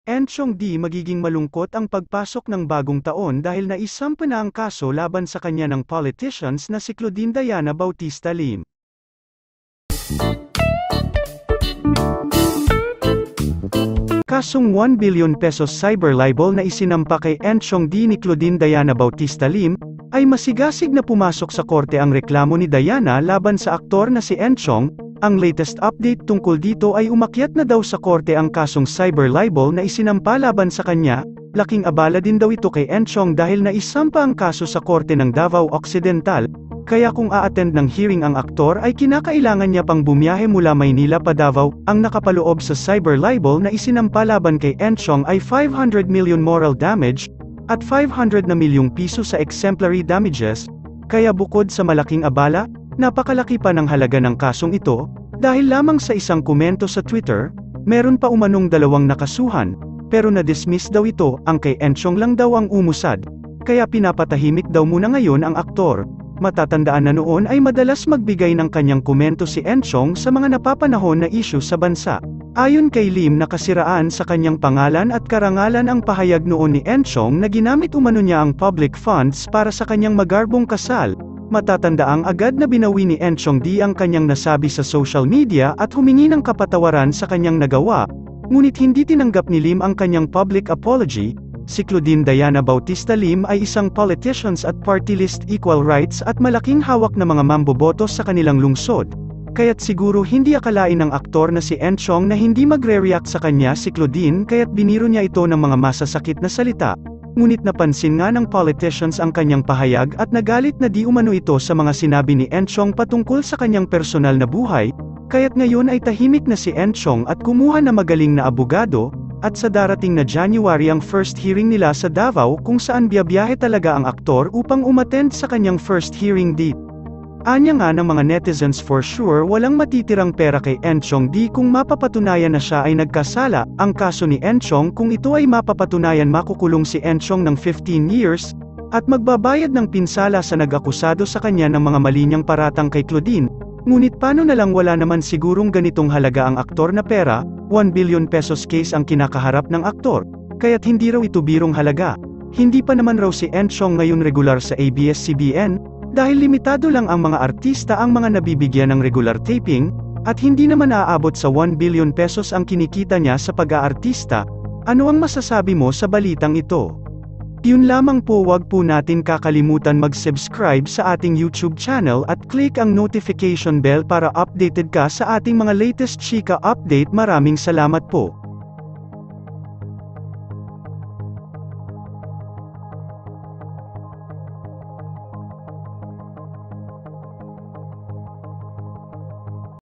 1 BILLION CASE na SINAMPA ni Claudine Diana Bautista Lim kay Enchong Dee Issue 1 Billion
enchong dee issue 1 billion,
enchong dee issue 1 billion case,
enchong dee 1 billion case,

PANO NG YARI ANG PANINIRA NI ENCHONG?